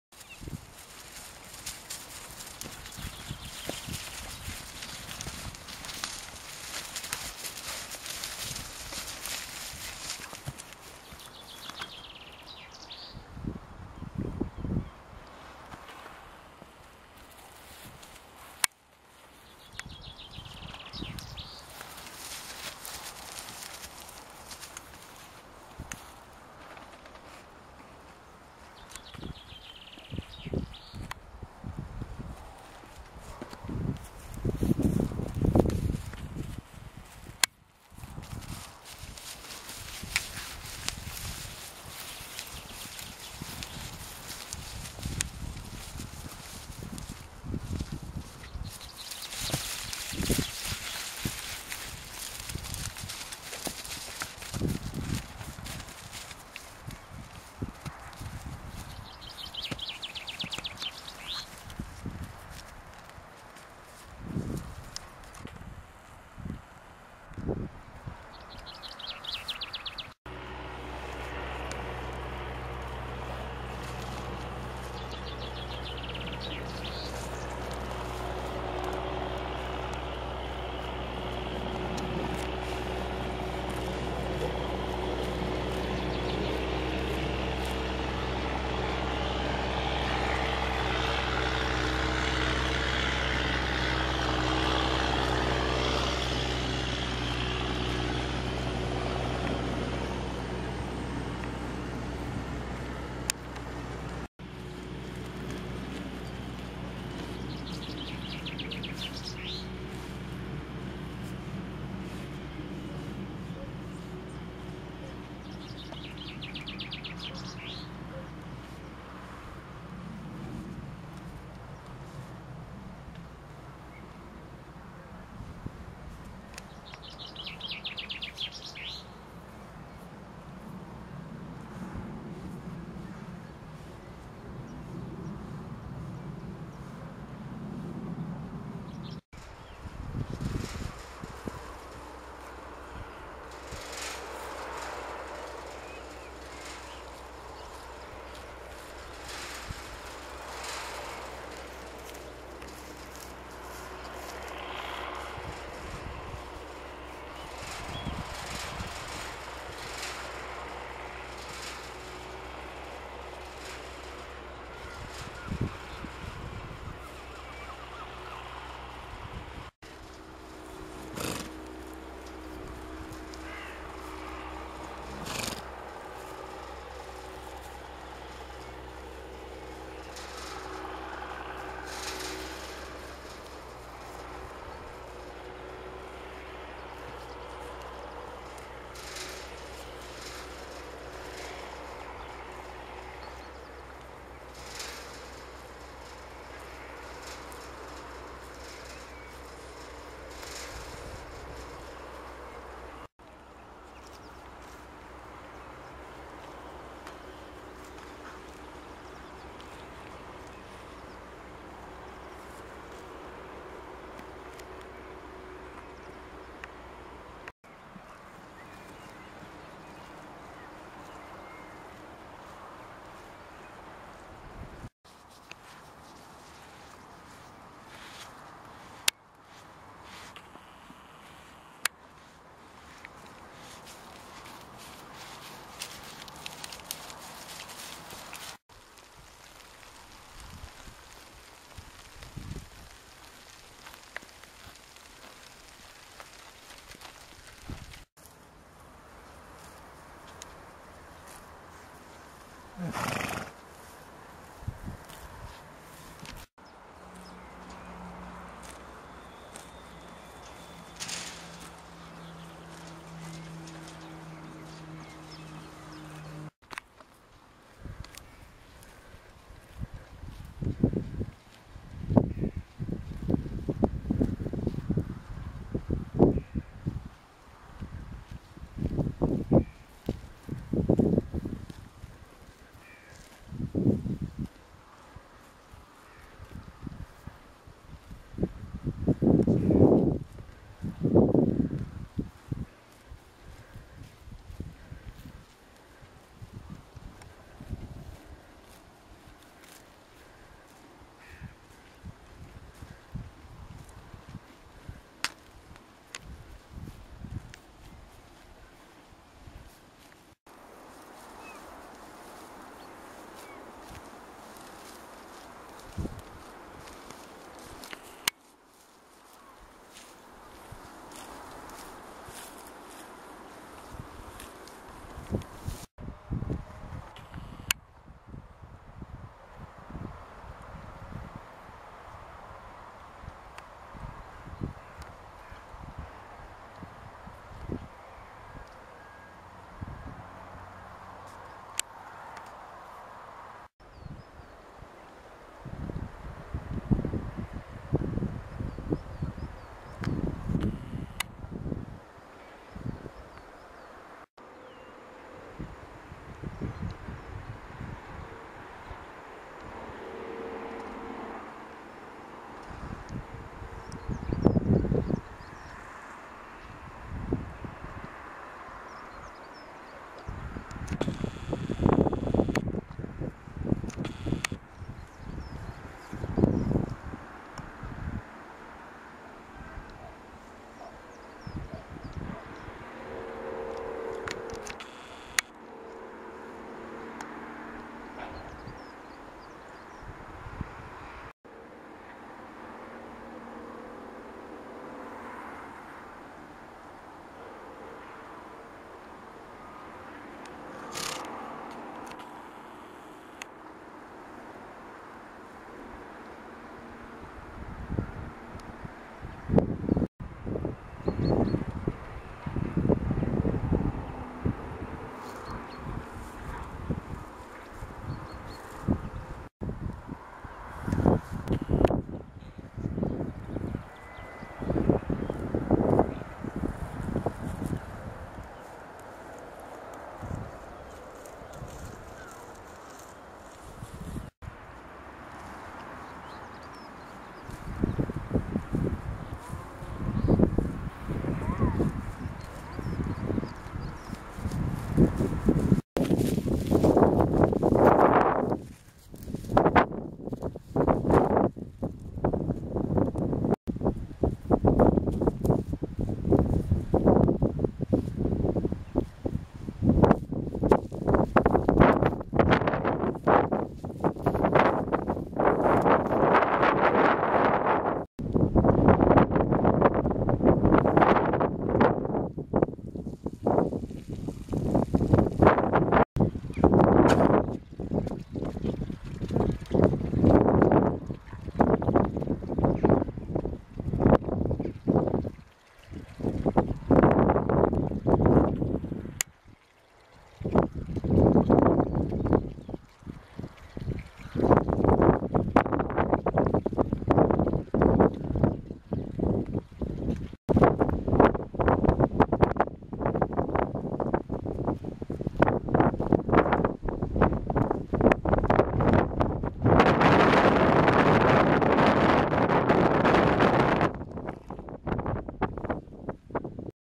Exmoor Ponies on North Berwick Law, April 2014
Happy Exmoor ponies, running, playing, grooming each other and scratching themselves  at North Berwick Law, April 2014